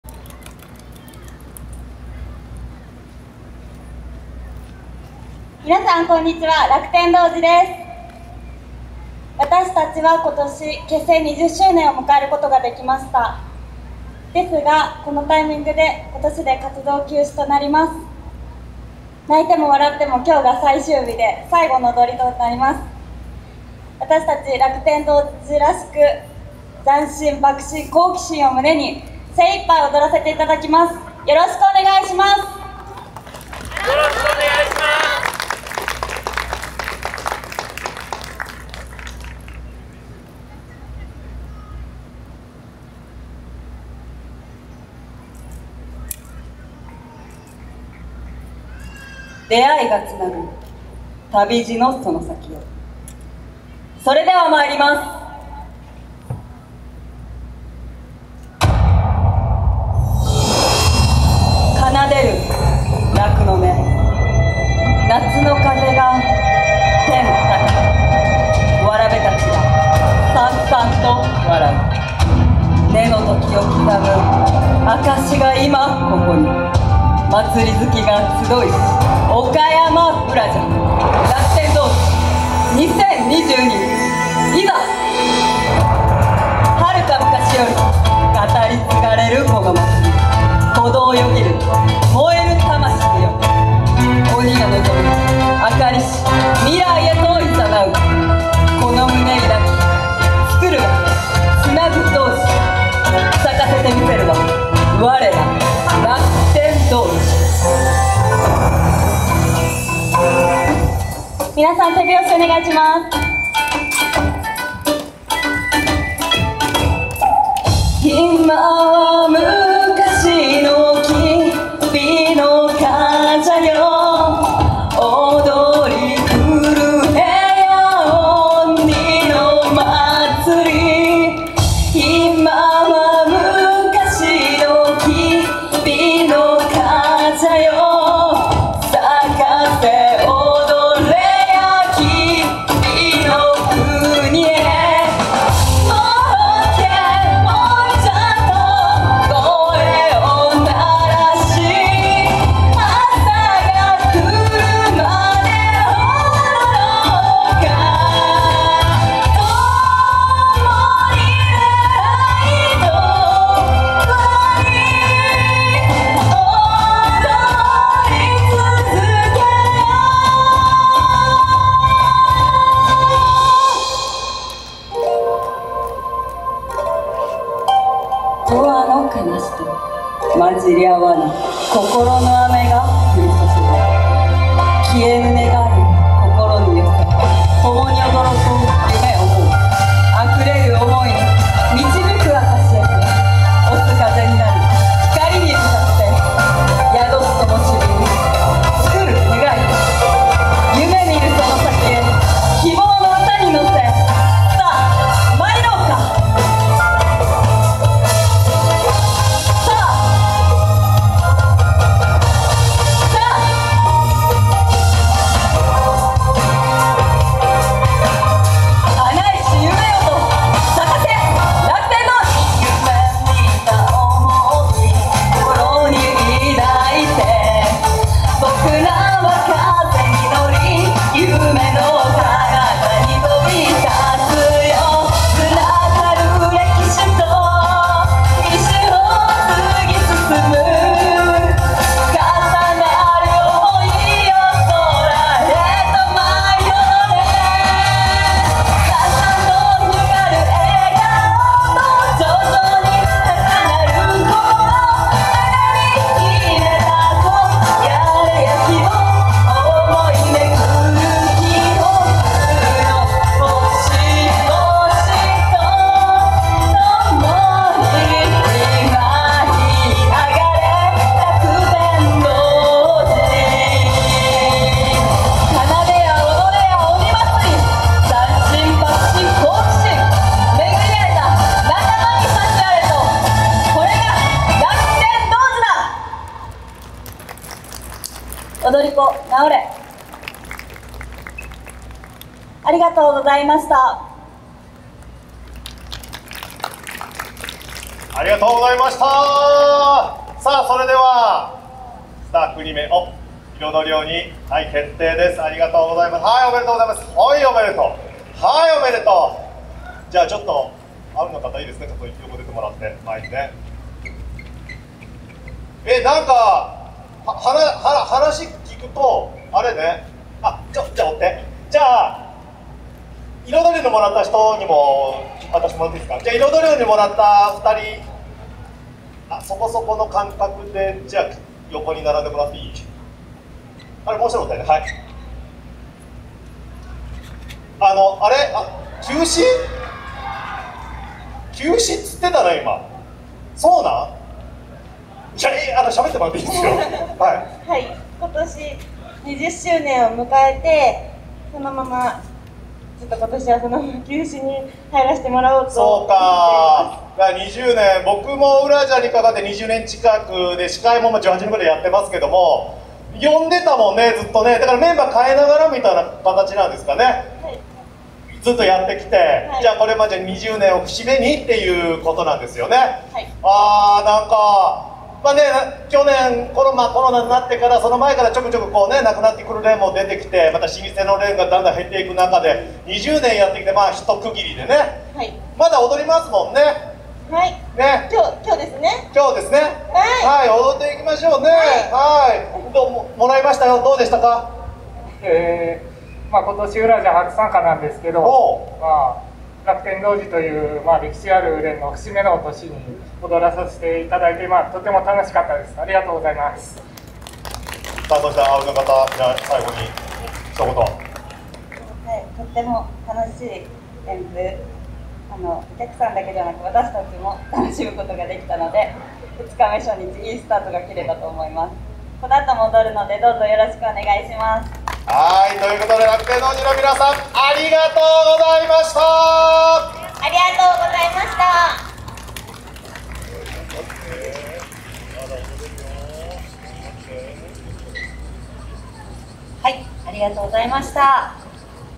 楽天童子 📍うらじゃ (下石井公園演舞場) 📅2022-08-21T13:20
2022年8月21日(日)、岡山県岡山市で開催された「うらじゃ」(下石井公園演舞場)での「楽天童子」さんの演舞です。